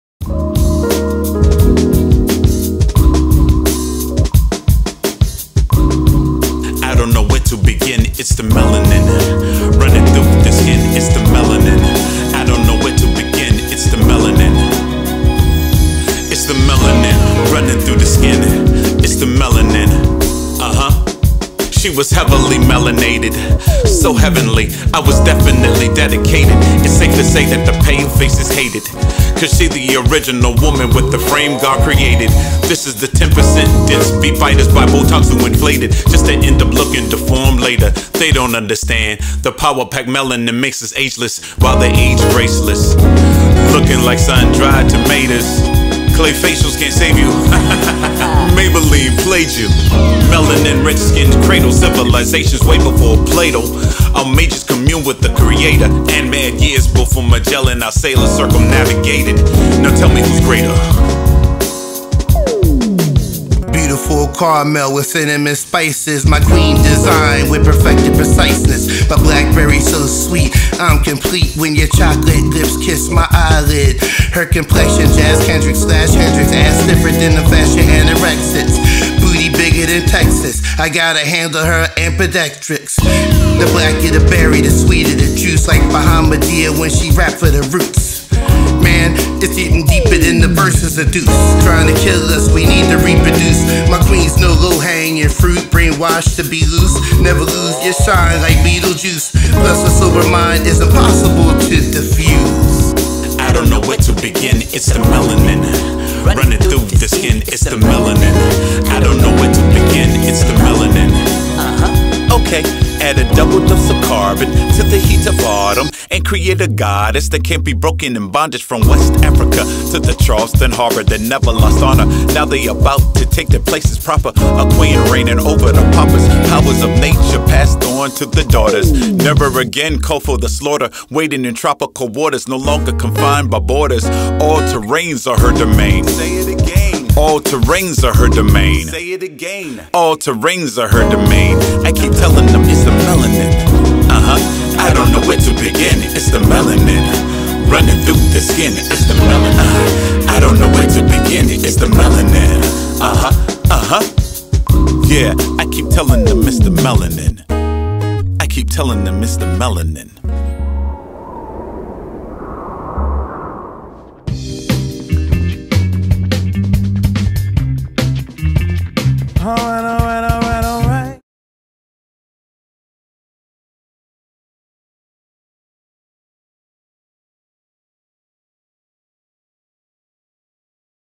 Melanin
Women of color have been imitated and emulated by all races without being given proper respect as the originators of culture. It from the womb-man that all mankind arises. And though I am a lover of all women of ALL races, I must pay homage to my first love: the Black mother. "Melanin" celebrates the women of my family specifically but also women of African diaspora globally. This video is a slice of life that shows what it means to me to be a Johnson from Liberty Hill in N. Charleston, SC, and it displays the absolutely BEAUTIFUL genes that I am blessed with. I love my mama and sisters and aunties and cousins and nieces. Special thanks to D'andrew Keasler-Bruce & Laurie Kellogg for arranging the models from the upstate.
LaBodegaMan FOREVER!